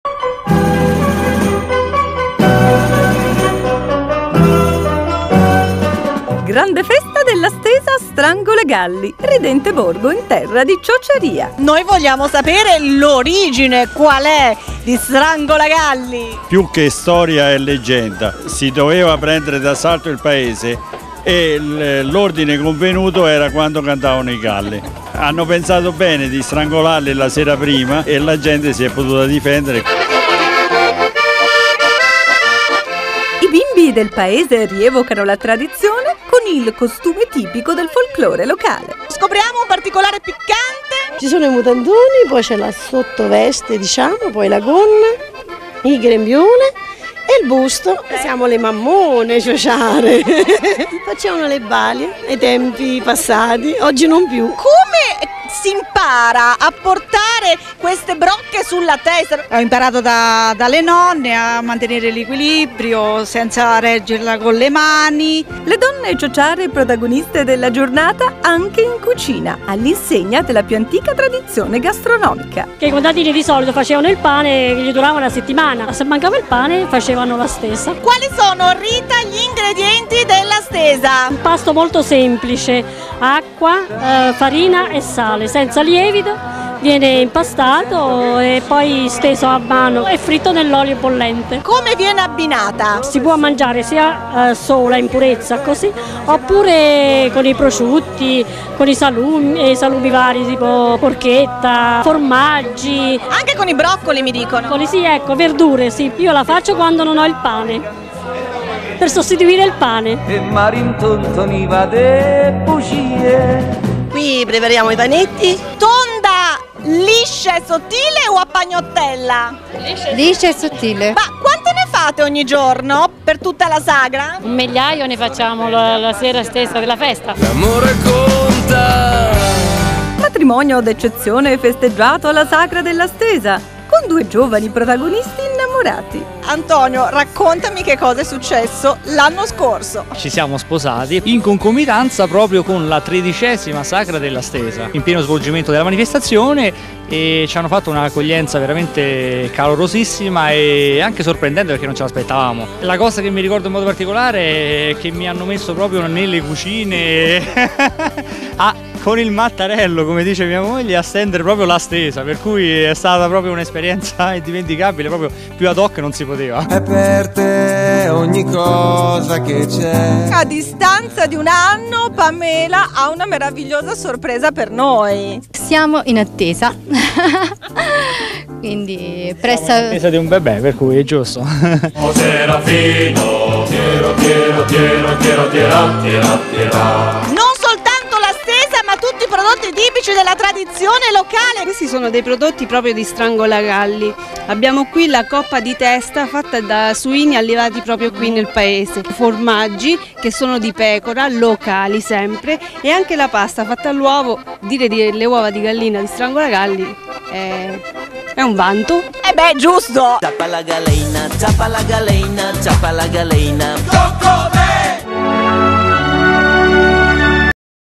Festa della Stesa Strangolagalli
Annuale appuntamento Gastronomico Tra Tradizione e cultura locale La festa della Stesa. Assolutamente da gustare, la stesa strangolagallese è impastata con solo farina e sale, aiutandosi con lacqua necessaria a ottenere un impasto omogeneo e piuttosto consistente. Si manipola a mano limpasto per circa 10 minuti.
Si suddivide in tanti pezzi grossi come 2 uova, per poi stenderli col mattarello in modo che formino delle sfoglie di più piccolo spessore possibile, per essere poi cotta in olio bollente.

Dopo la cottura, la fragrante stesa viene ripiegata su se stessa, sino a formare una mezzaluna e vengono cosi servite, farcendole con prelibato prosciutto crudo o cotto, con formaggio oppure con un impasto di formaggio cremoso, ed ancora con broccoletti, mortadella, coppa e (mai porre limiti alla fantasia!) Molti la gradiscono, caldissima e senza alcun ripieno.